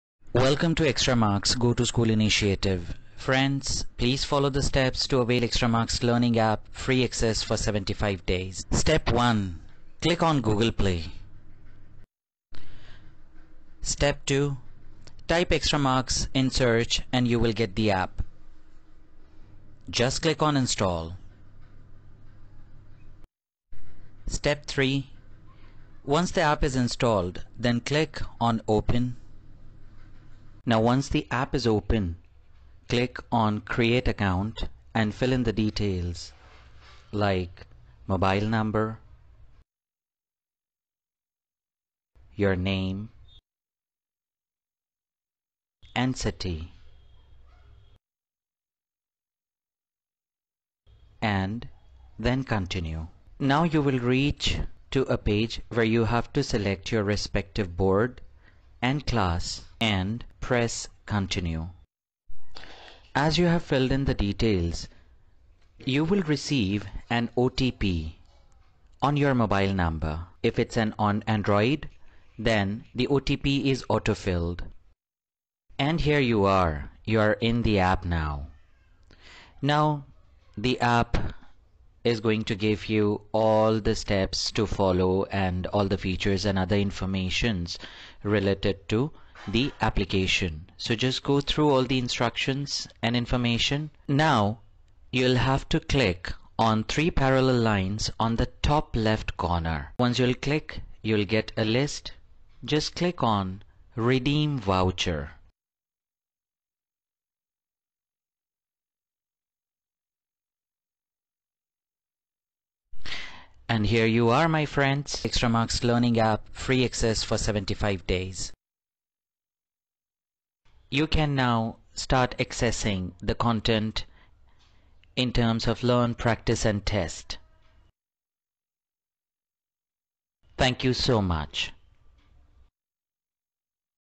Extramarks App Download Steps. Stay home Stay safe.
This video help a user to understand the steps to download the Extramarks app for your Reference